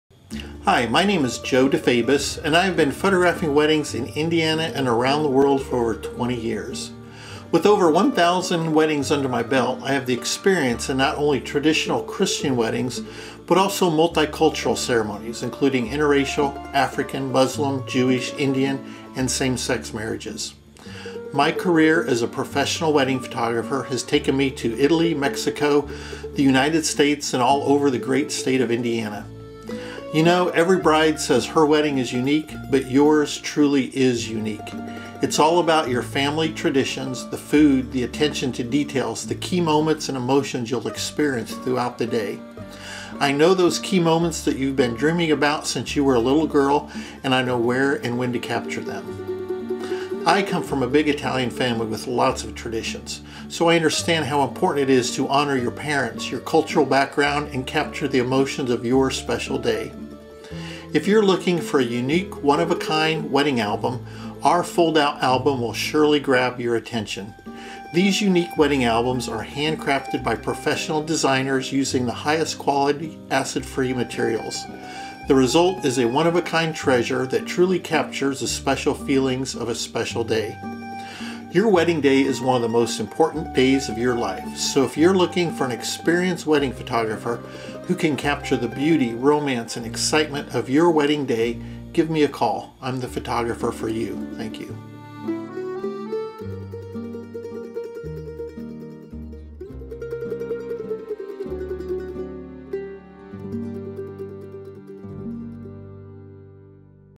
Indianapolis wedding photographer
Indianapoolis wedding photographer Joe DeFabis talks about his experience and approach to wedding photogrphy.
2940 W 39th Street
Indianapolis, IN 46228
Fo rmore information visit us at;